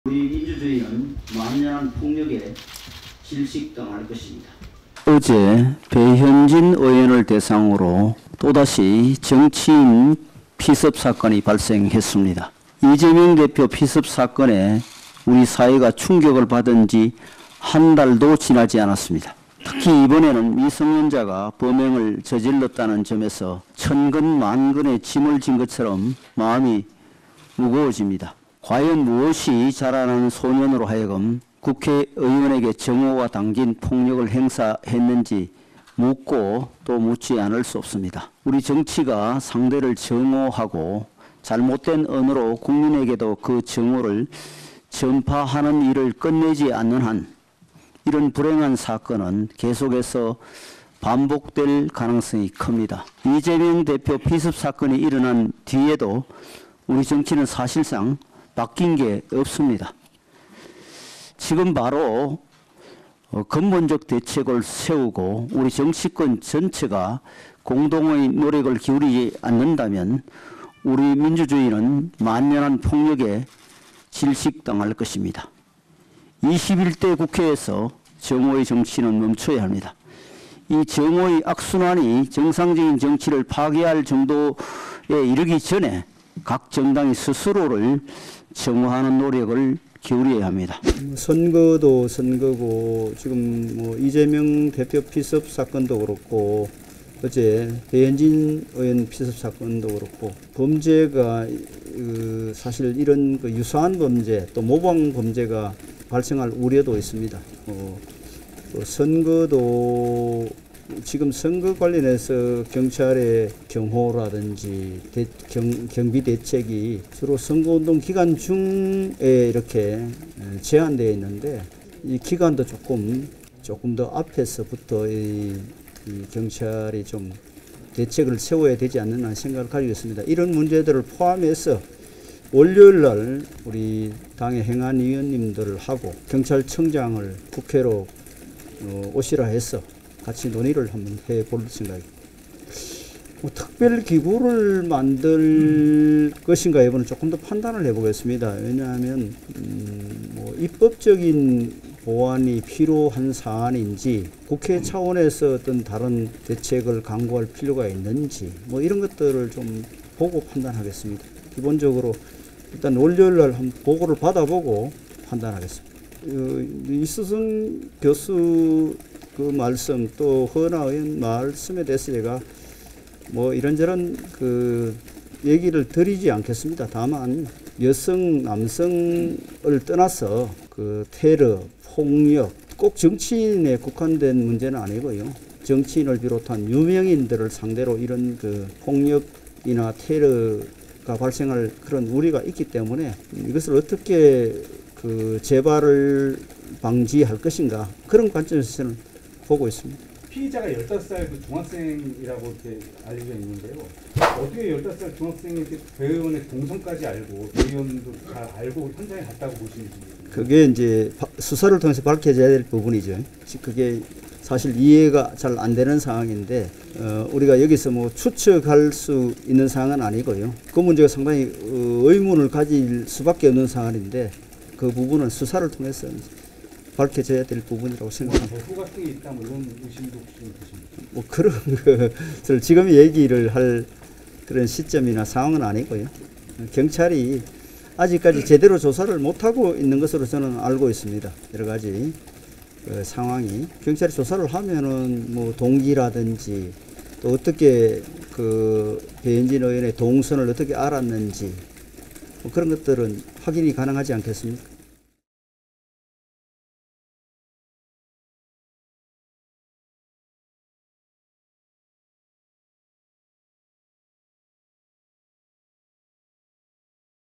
[현장영상] "배후 있다고 보나" 윤재옥, 훅 들어온 '배현진 습격 사건' 질문에.. / JTBC News
0:00 국민의힘 원내대책회의 - 윤재옥 원내대표 발언
1:31 "논의 중인 안전 대책 있는지?"
2:22 "관련 특별 기구 설립 계획은?"
2:46 이수정 교수 '여성이라서 피해자 됐을 수도' 발언 관련
3:20 "15살 중학생 피의자가 어떻게 동선 확인을?"
3:48 "사건의 배후가 있다는 의구심 드는지?"

오늘(26일) 윤재옥 국민의힘 원내대표는 배현진 의원 피습 사건과 관련해 "증오의 정치를 멈춰야 한다"며 각 정당에 자정을 촉구했습니다.